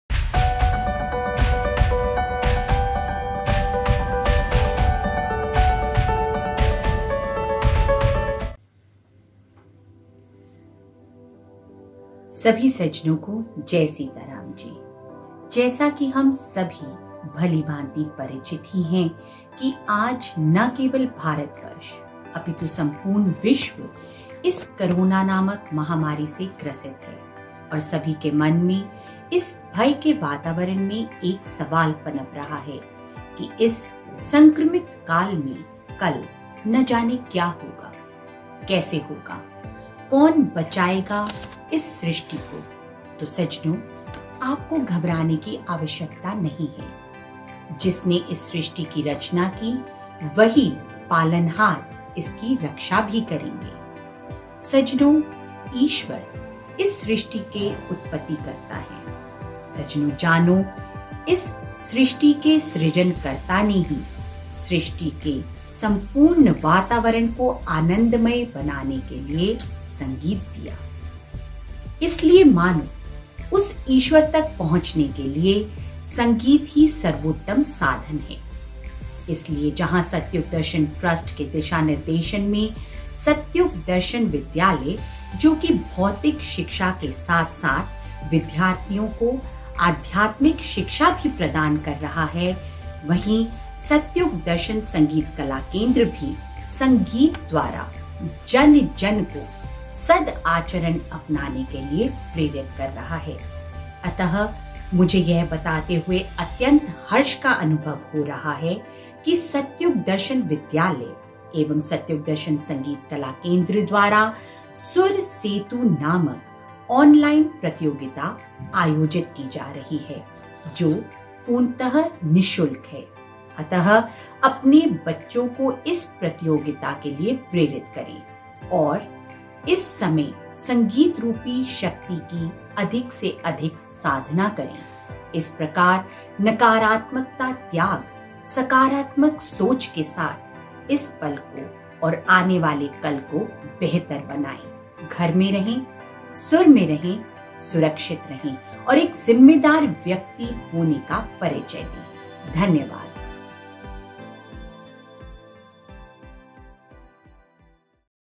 SUR SETU ONLINE TALENT HUNT COMPETITION MESSAGE BY CHAIRPERSON OF SATYUG DARSHAN SANGEET KALA KENDRA
MOTIVATION GIVEN BY CHAIRPERSON OF SATYUG DARSHAN SANGEET KALA KENDRA(Smt.ANUPAMA TALWAR)